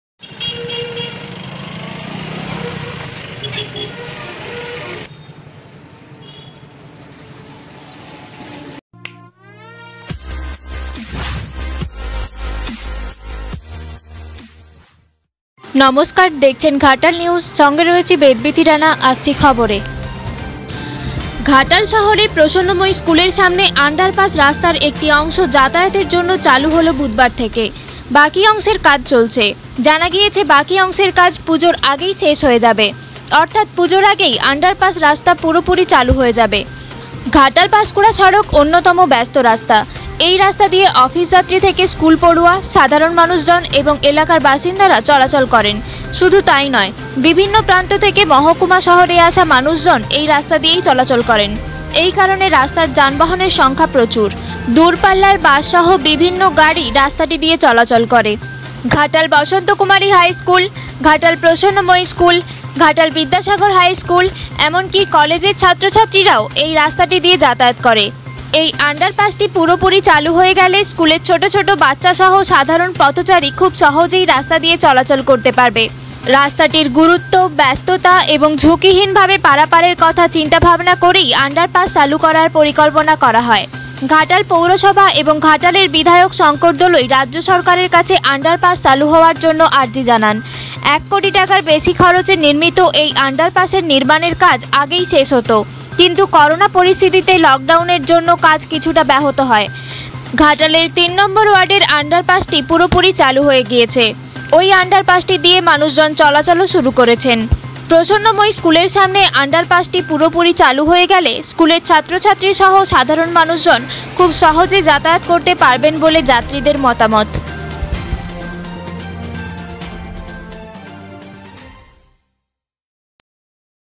ঘাটাল প্রসন্নময়ী স্কুলের সন্নিকটে অান্ডারপাশ রাস্তা তৈরী
ঘাটাল প্রসন্নময়ী স্কুলের সন্নিকটে অান্ডারপাশ রাস্তা তৈরী হচ্ছে, একদিকের রাস্তা তৈরী হল এবং অন্যদিকের রাস্তা তৈরীর কাজ শুরু হয়েছে।